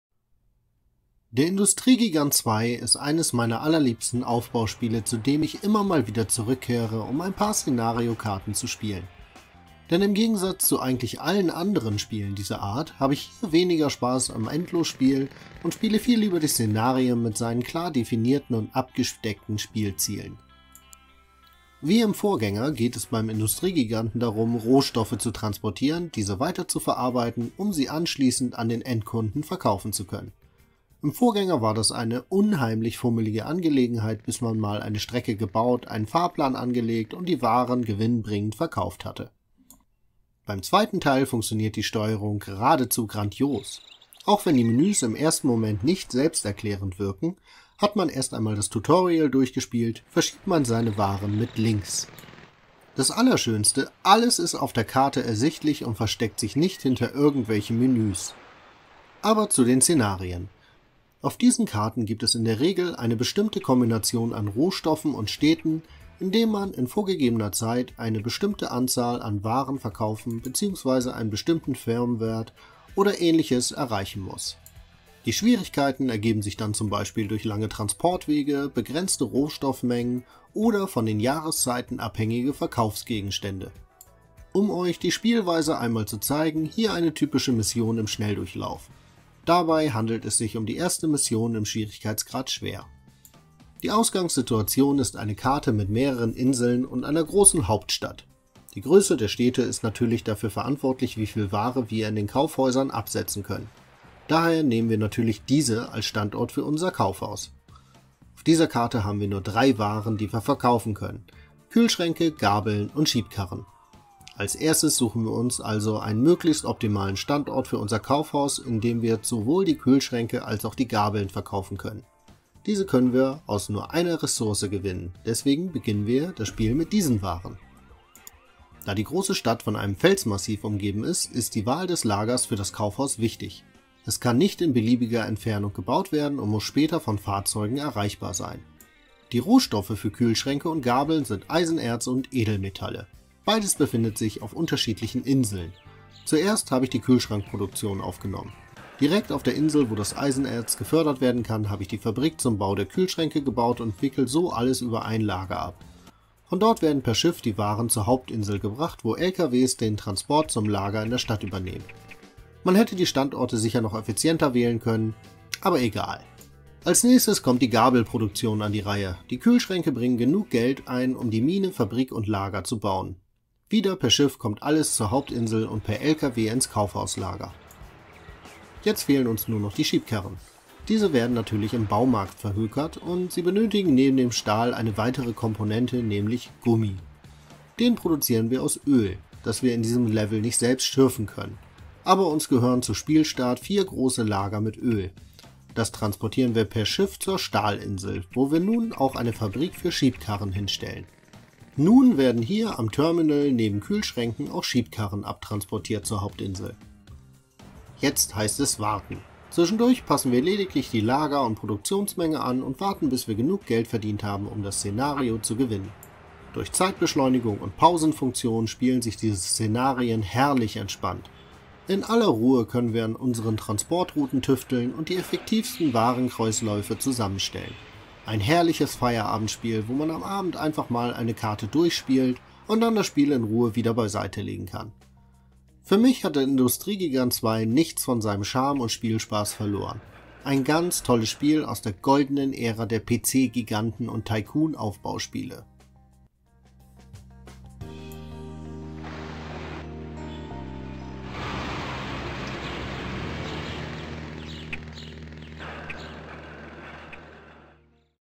Rückblick - Der Industrie Gigant 2 - 21.06.2002
Eines der besten Aufbaustrategiespiele seiner Zeit. Was ich heute vom Spiel halte seht ihr im Video.